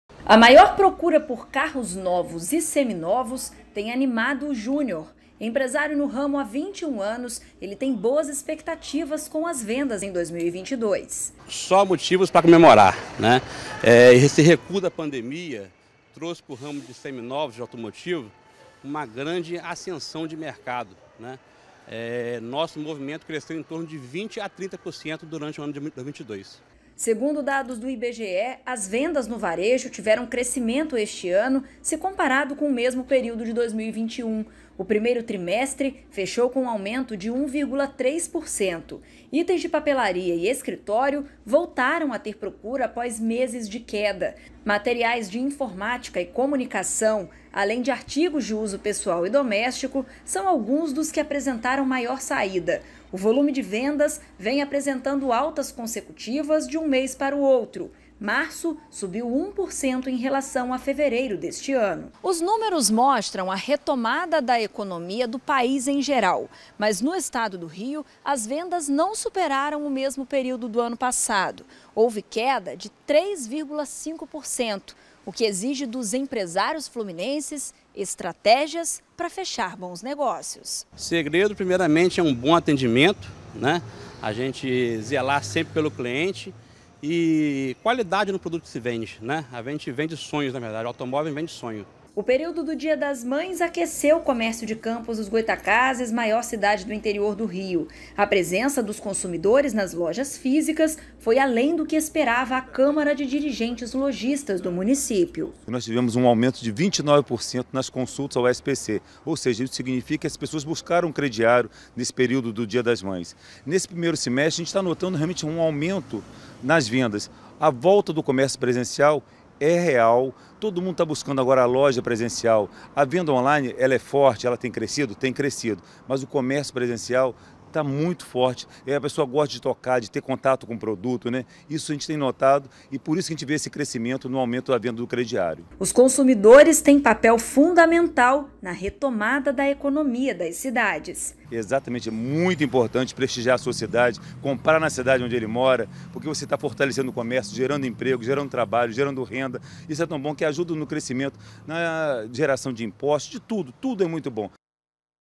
Início de ano positivo para empresários do varejo
O ano de 2022 está melhor para grande parte dos empresários do varejo. Com a retração da pandemia e a volta de todas as atividades presenciais, alguns setores do comércio apresentam bons resultados.